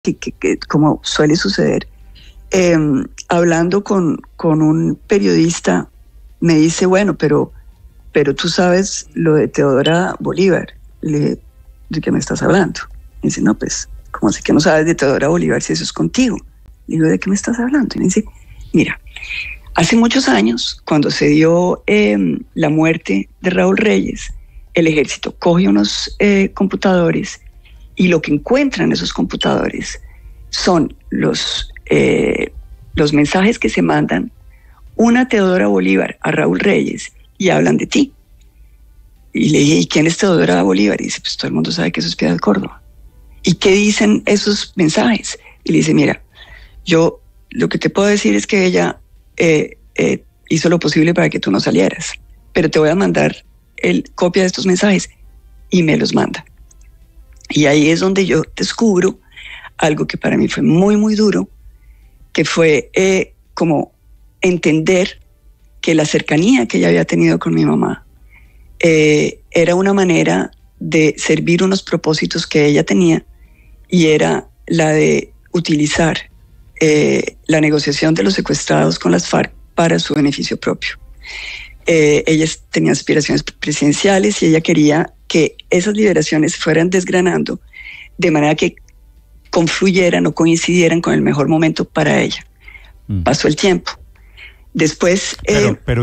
“Piedad Córdoba utilizaba negociación de secuestrados para su beneficio”: Ingrid Betancourt
De acuerdo con el relato de Betancourt, en medio de una conversación con un periodista, tiempo después de su rescate, supo de los computadores y de los correos que identificaban a Piedad Córdoba como ‘Teodora Bolívar’.